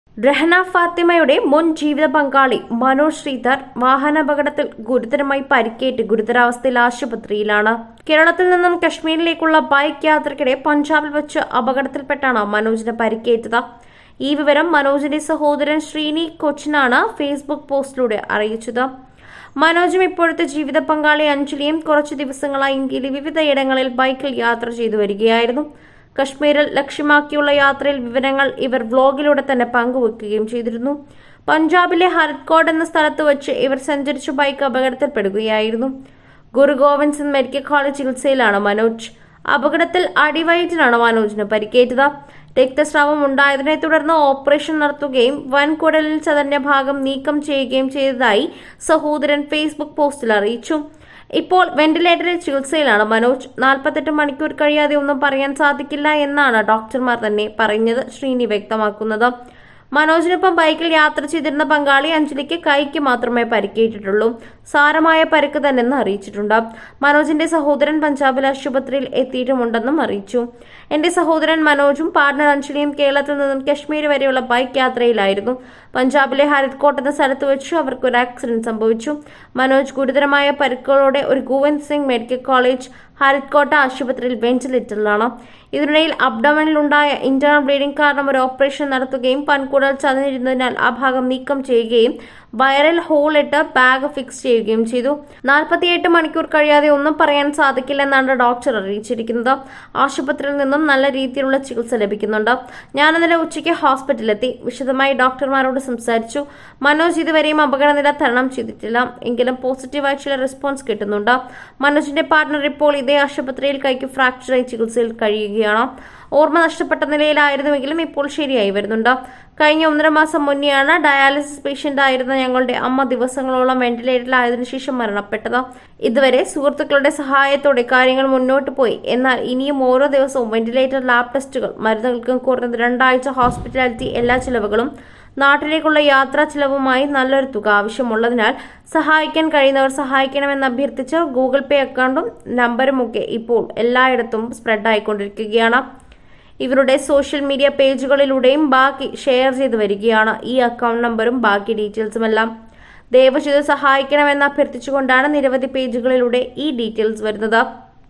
രഹന ഫാത്തിമ യുടെ ആദ്യ ഭർത്താവ് അപകടനിലയിൽ വെൻറിലേറ്ററിൽ l Manoj K Sreedhar
രഹനാ ഫാത്തിമയുടെ ആദ്യഭർത്താവ് അപകടനിലയിൽ..
സഹായമഭ്യർത്ഥിച്ചു കൂട്ടുകാർ..